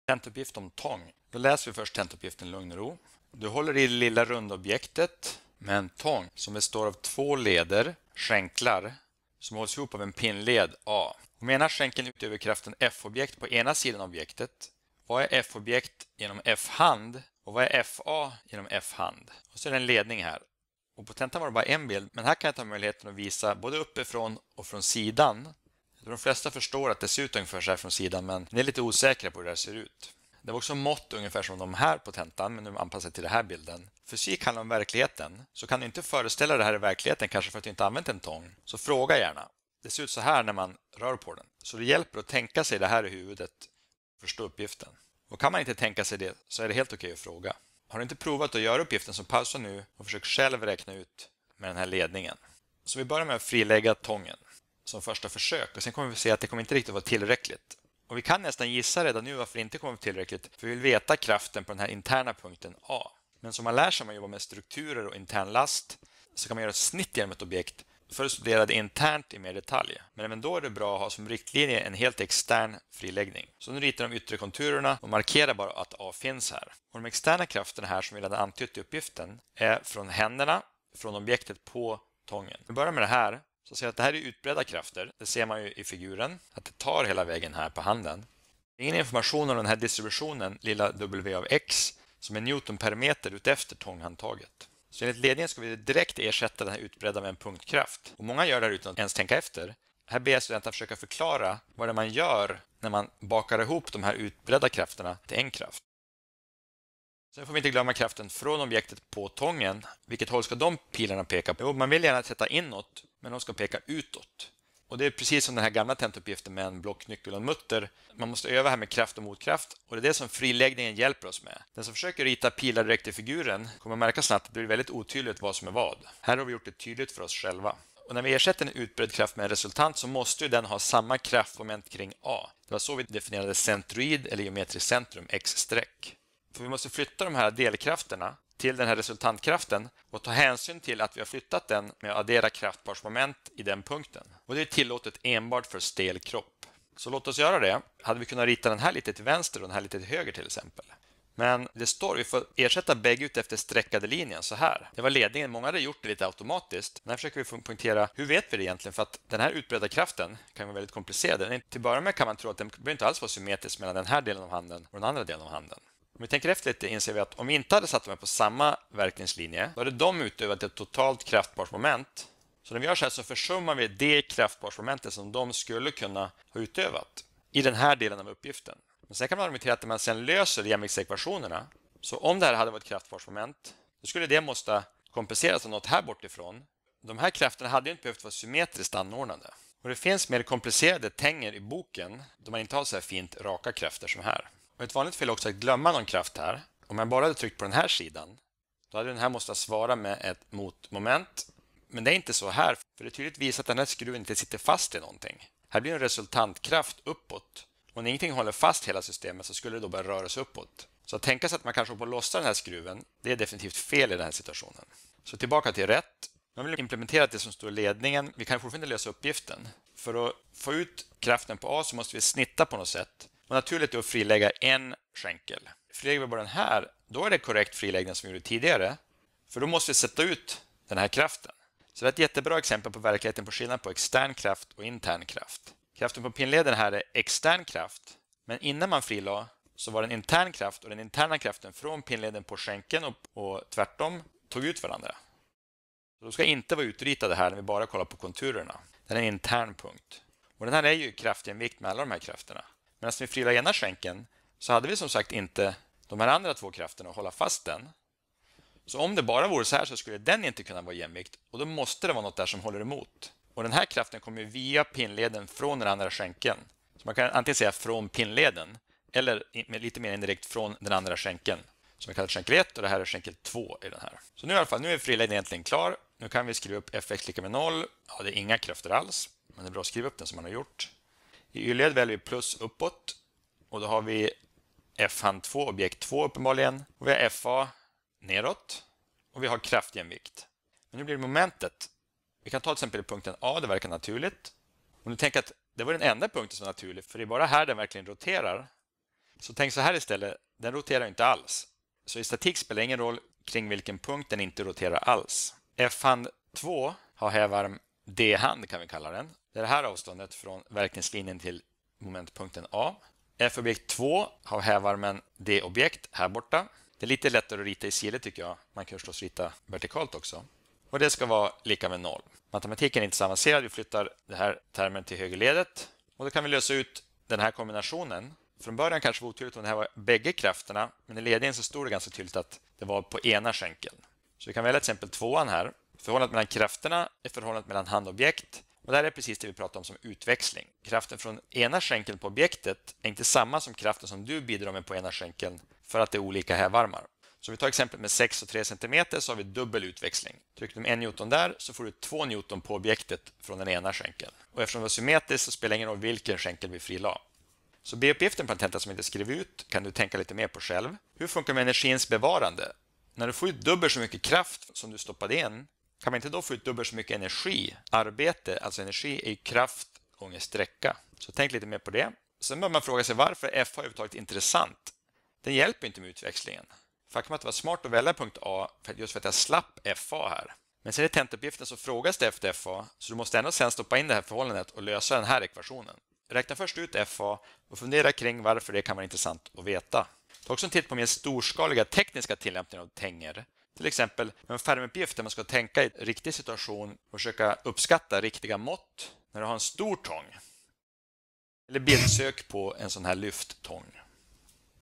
Tentauppgift: tång
Det är väldigt lite matematik i den här deluppgiften på en tenta, men desto mer fysik: förflyttningslagen, utbredd kraft, kraftparsmoment, momentjämvikt, intern/extern kraft och särskilt Newtons 3:e lag (kraft/motkraft).